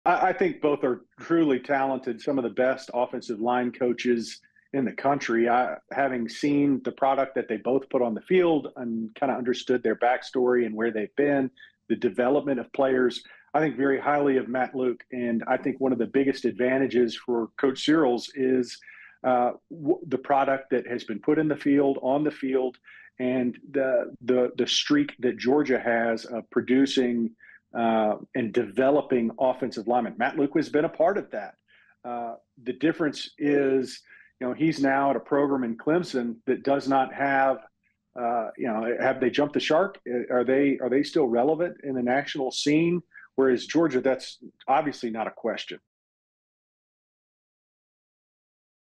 Has Clemson jumped the shark?
As UGA battles Clemson for 5-star offensive line recruit David Sanders, UGA All-American Jon Stinchcomb points out the differences between the Dawgs & Tigers programs.